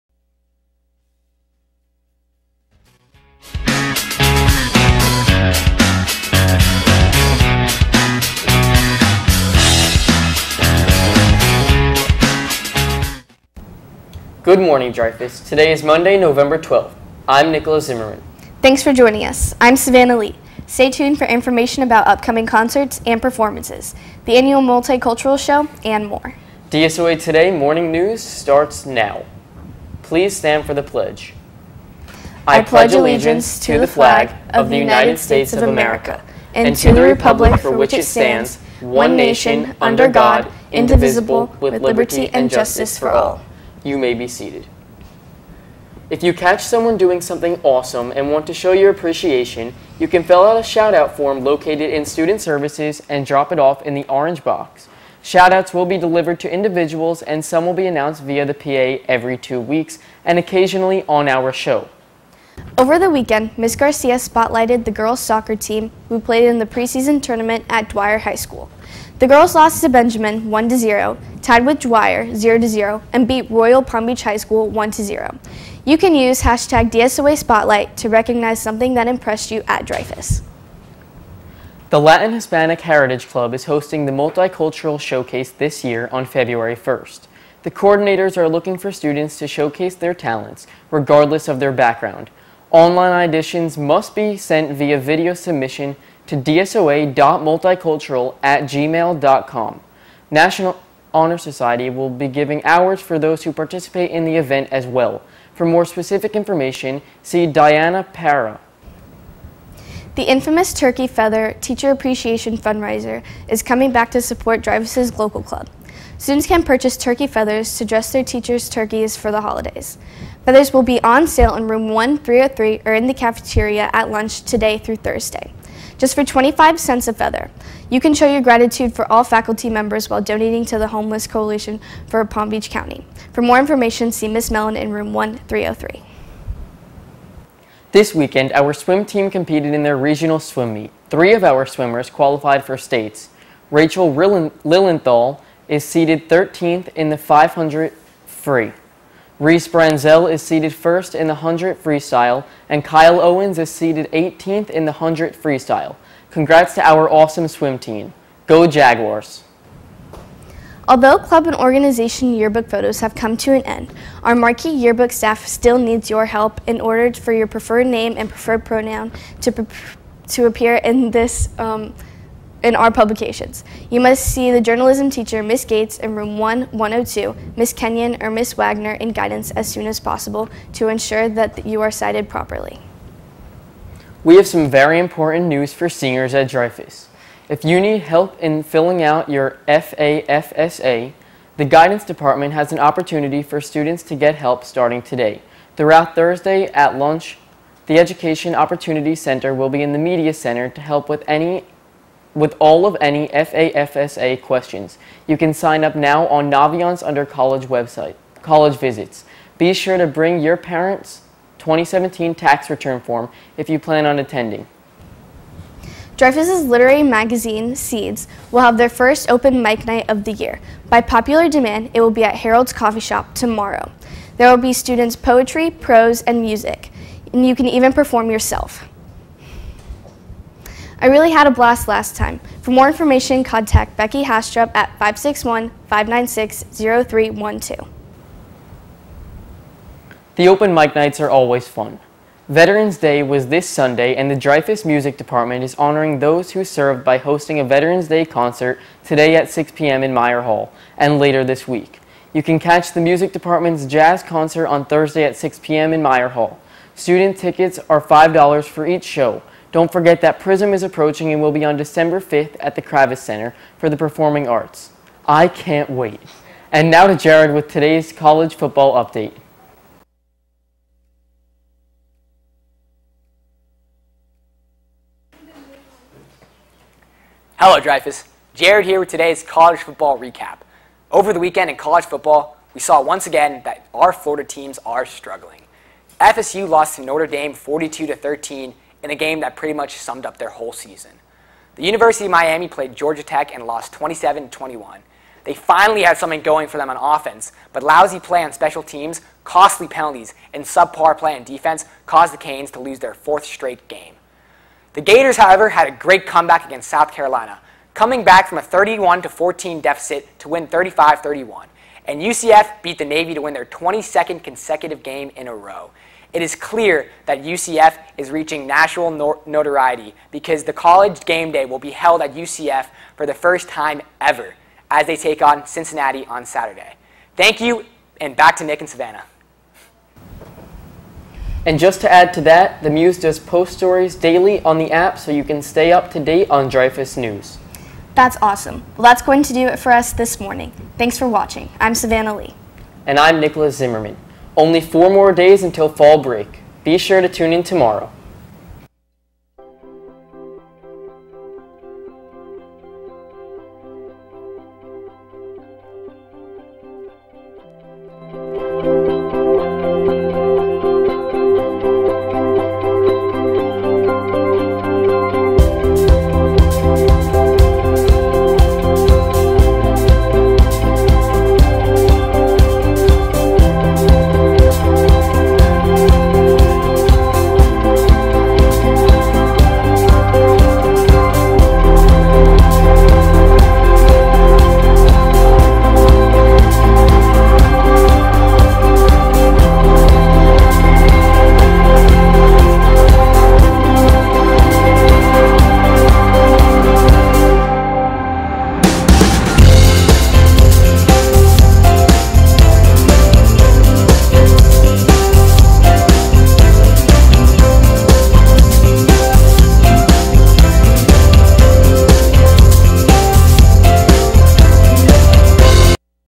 DSOA TODAY 11-12-2018
Featuring:
Anchors- Nikolas Zimmerman and Savannah Lee
Segment Host- Jared Civin
Crew:
Teleprompter Operator- Jasper Weinberg
Cameras- Taylor Haines, Sean Horan and Julian Riley
Floor Director- Vanessa Benitz
Technical Director- Natalie Cohen
Assistant Director- Ken Tanner
Audio- Jessica Gomez

Veteran's Day Package- Nik Zimmerman